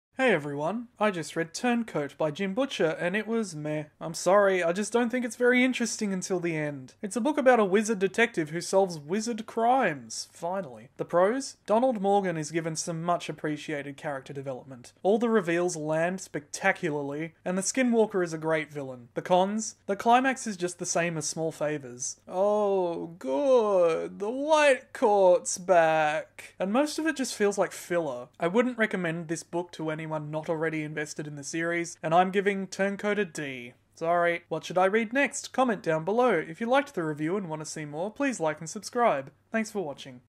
Shagnasty's Great, But... (Turn Coat Review)
Time Trial Book Reviews is a show where I review books as quickly as I can.

Today's book is Turn Coat by Jim Butcher.

LEAVE A READING SUGGESTION IN THE COMMENTS BELOW!